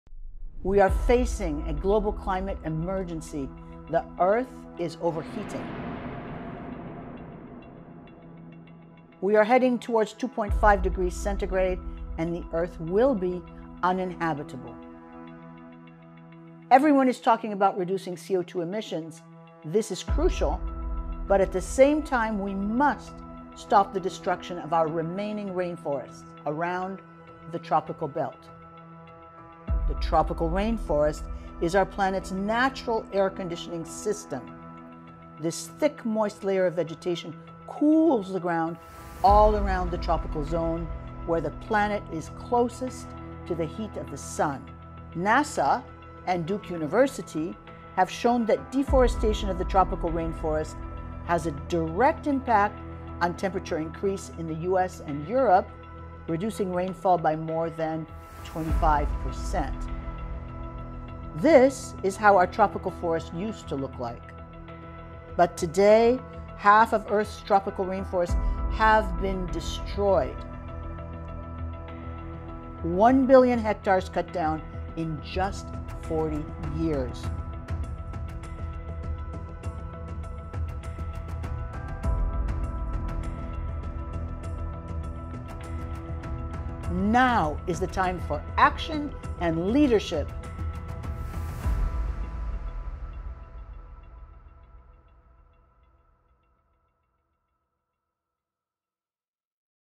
Climate Crisis 2022
We are facing a global climate emergency.
We are heading toward a 2.5 C temperature rise, making much of the Earth will be uninhabitable.
The answer? Direct protection of forests on the ground, especially where it matters: tropical forests which are important for carbon storage and biodiversity.